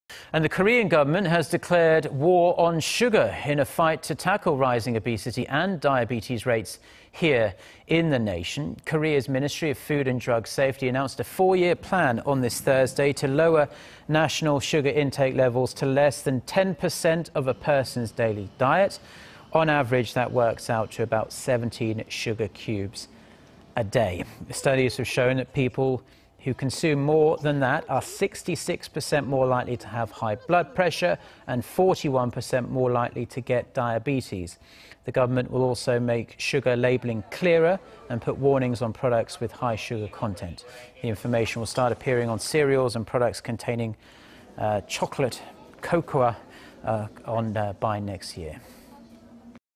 Korea announces 'war on sugar' to tackle rising obesity and diabetes rates
정부 '설탕과 전쟁' 선포…하루 각설탕 16.7개 이하 목표
The Korean government has declared a war on sugar, in a fight to tackle rising obesity and diabetes rates in the country.
Korea's Ministry of Food and Drug Safety announced a four-year plan on Thursday to lower national sugar intake levels to less than ten-percent of a person's daily diet.
On average, that works out to about 17 sugar cubes a day.
Studies have shown that people who consume more that that are 66-percent more likely to have high-blood pressure and 41-percent more likely to get diabetes.
The government will also make sugar labeling clearer and put warnings on products with high sugar content.
The information will start appearing on cereals and products containing cocoa by next year.